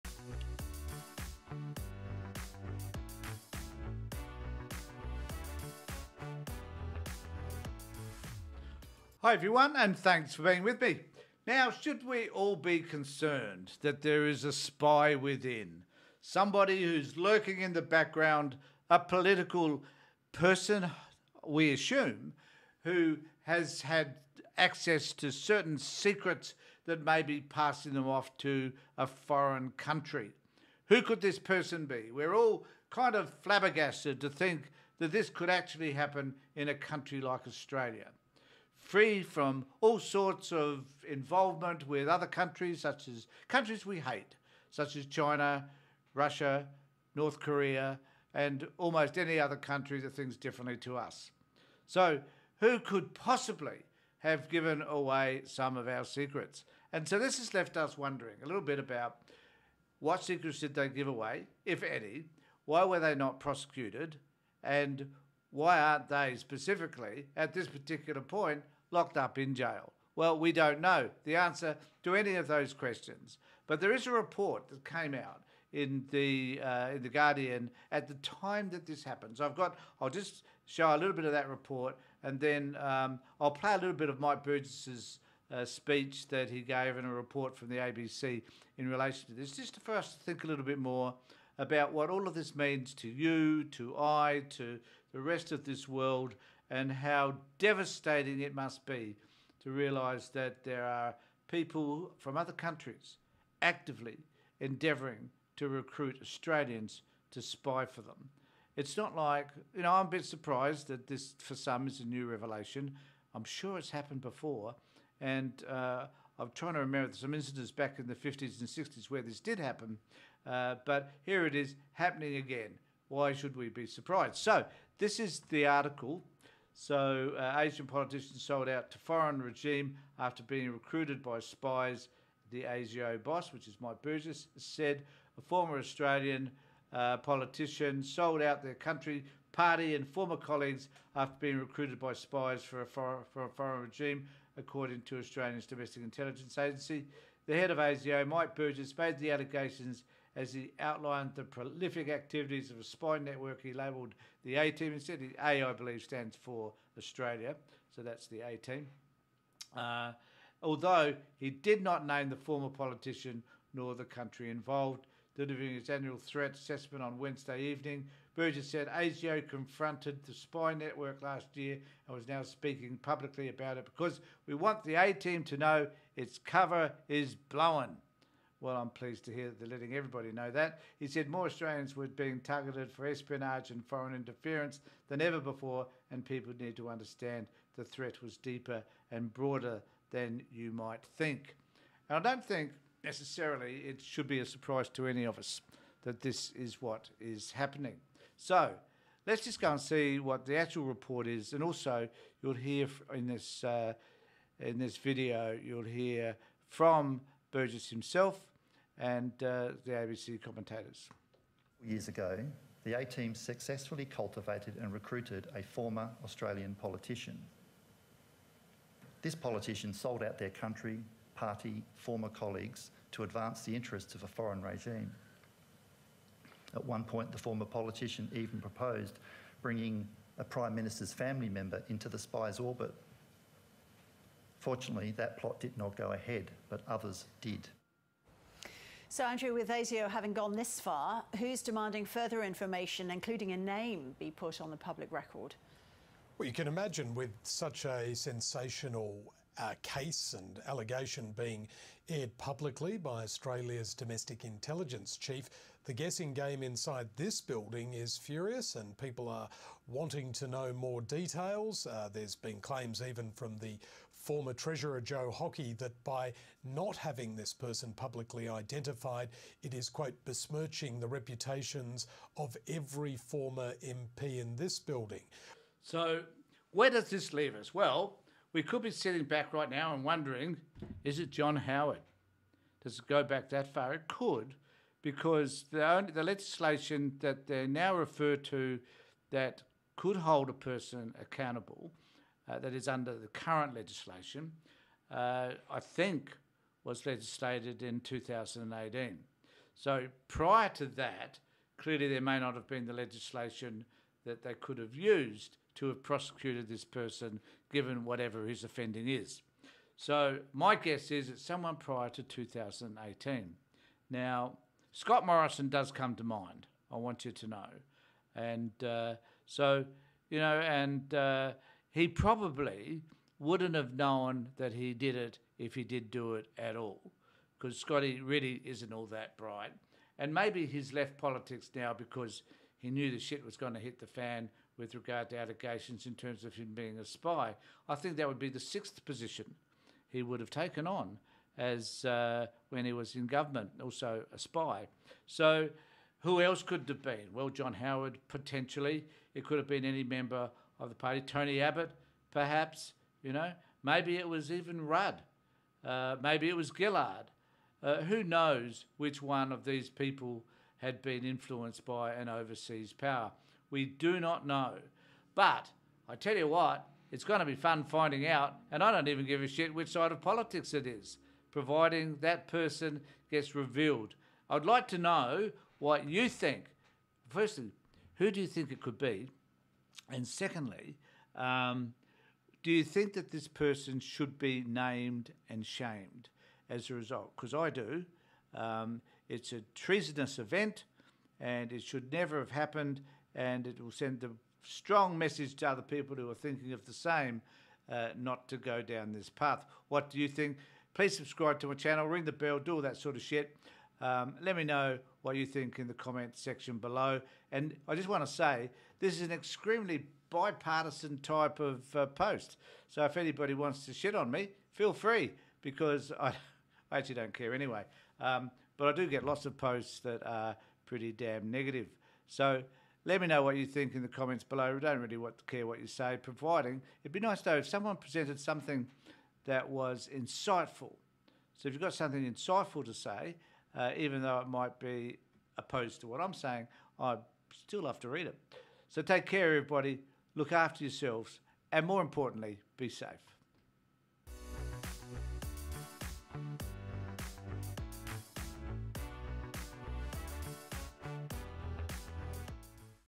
The Spy Within
The question everyone is asking is who is the politian who has been spying? We all need to know. Will be interested in any ideas you may have.